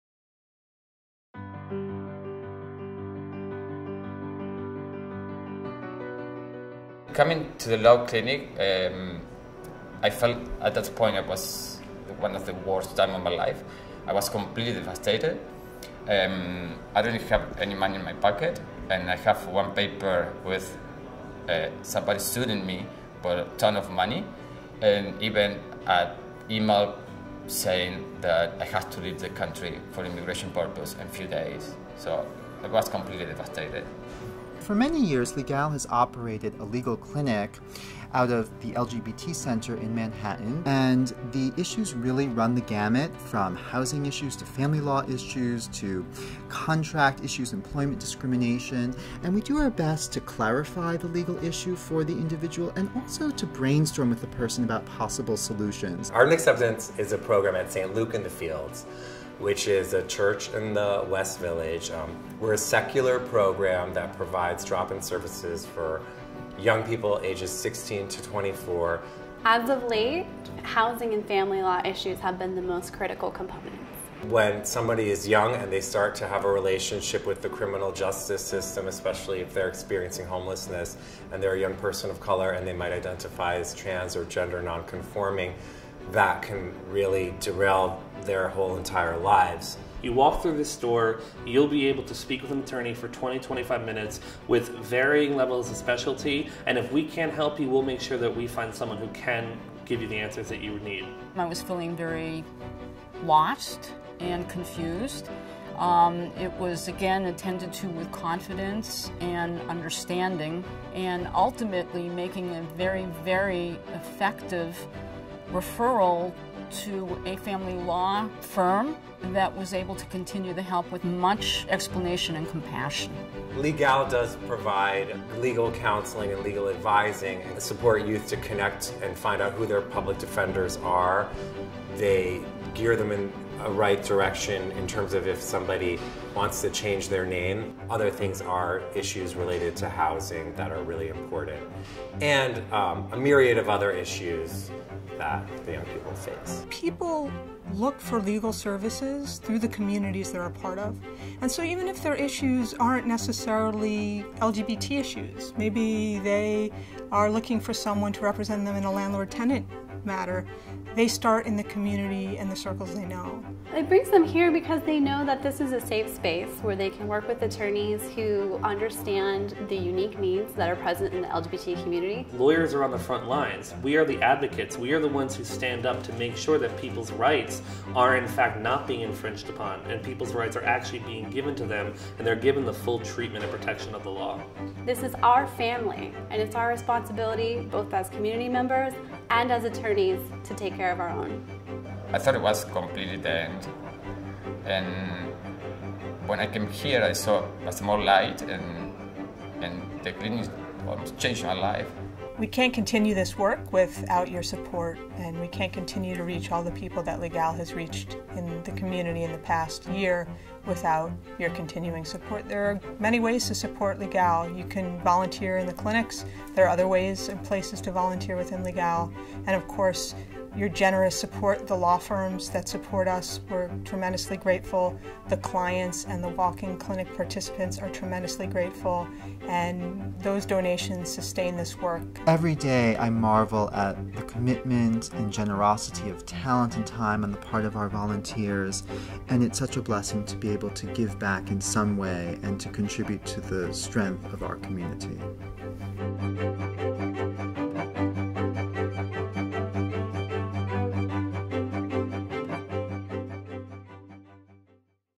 LeGaL's Legal Services Programs
The LGBT Bar Association of Greater New York (LeGaL) believes that the true test of any community is the extent to which it takes care of its members in times of collective struggle for justice, as well as in the seemingly ordinary struggles of our daily lives.  To that end, LeGaL provides meaningful legal services which offer holistic consultations, referrals, and representation, where possible, to members of our community.